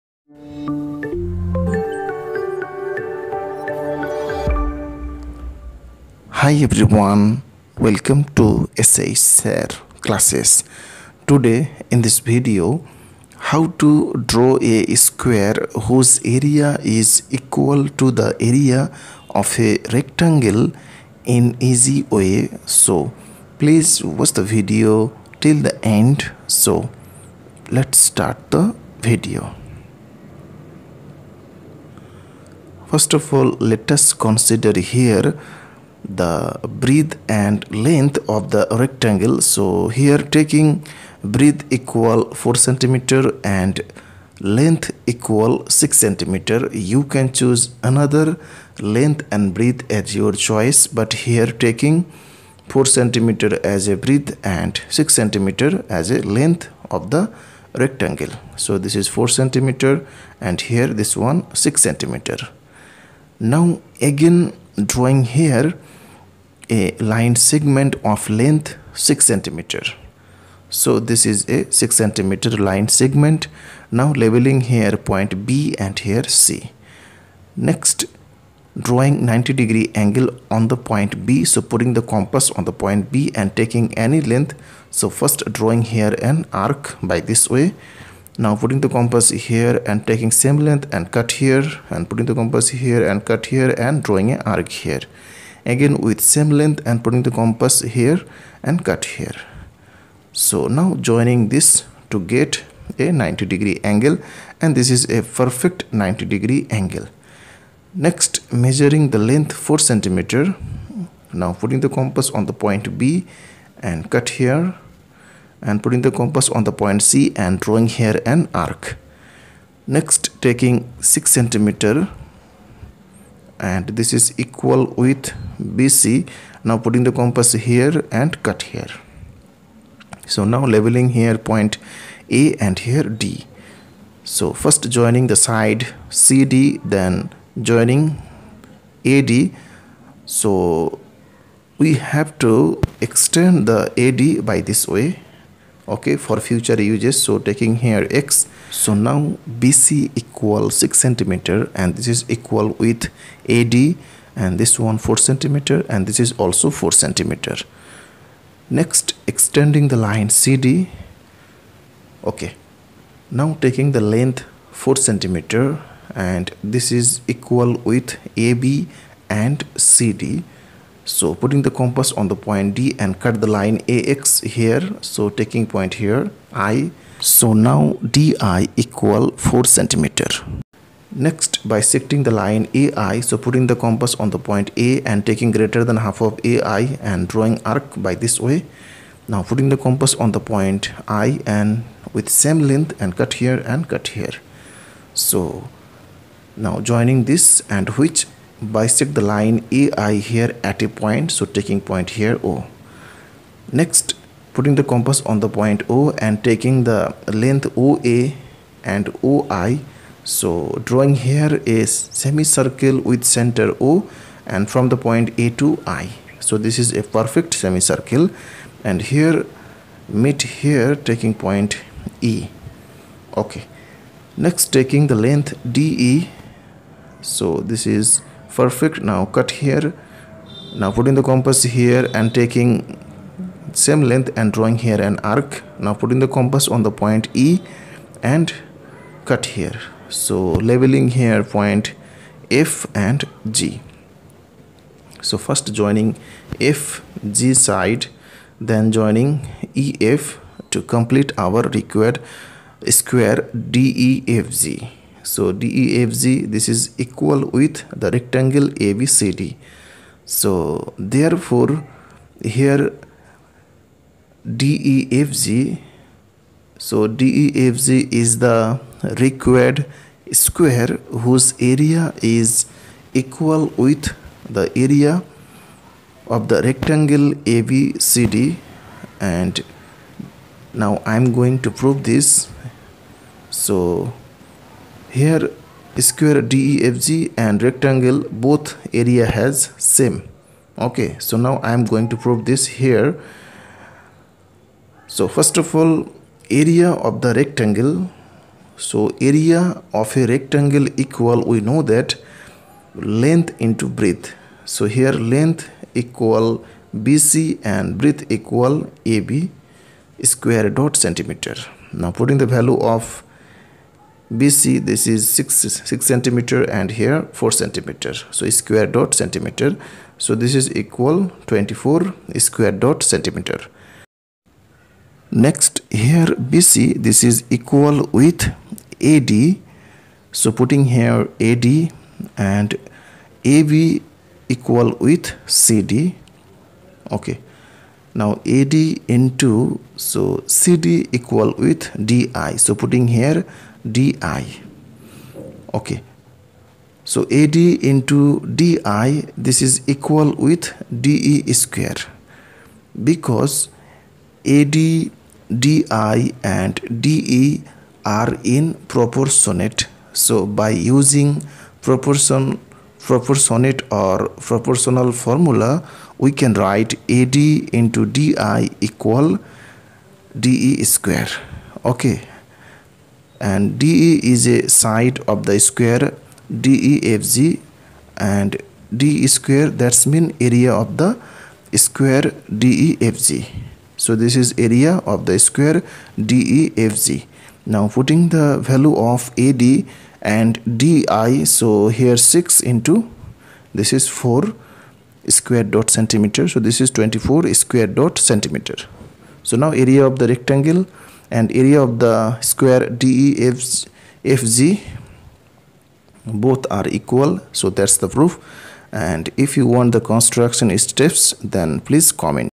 How to draw a square equal in area to a rectangle.
In Easy Way how to draw a square whose area is equal to the area of a rectangle. Geometrical Construction. @SHSIRCLASSES.